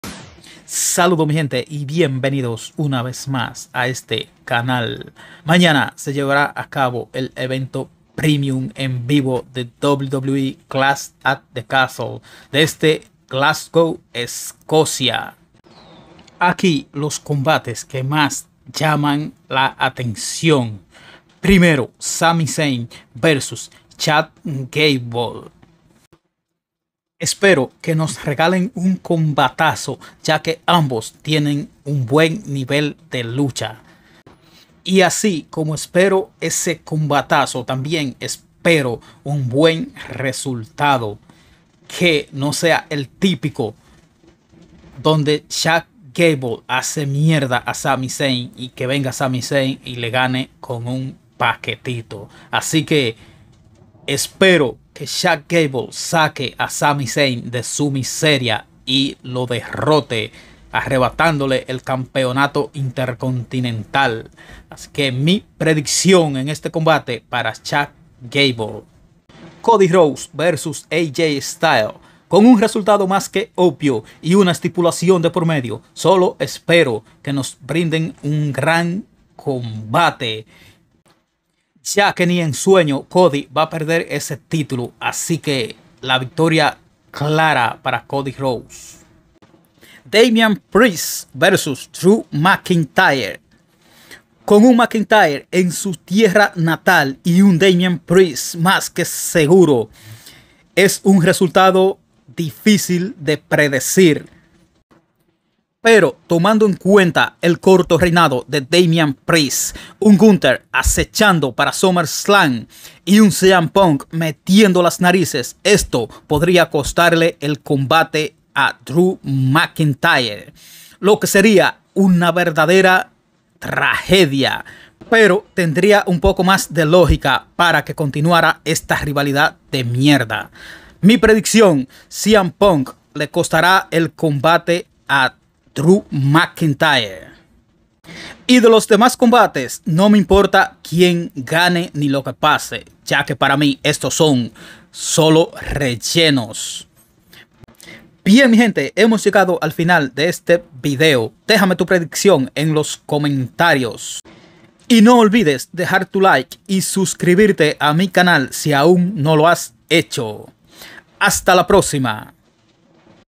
¡Predicción: Los combates más emocionantes en WWE Clash at the Castle de 2024!
Bienvenido a nuestro canal para una vista previa exclusiva del evento Clash at the Castle de WWE en vivo desde Glasgow, Escocia. Únase a nosotros mientras profundizamos en los enfrentamientos esperados, incluidos Sami Zayn contra Chad Gable, Cody Rose contra AJ Styles y Damian Priest contra Drew McIntyre. Sumérgete en nuestras predicciones y comparte tus pensamientos en la sección de comentarios a continuación.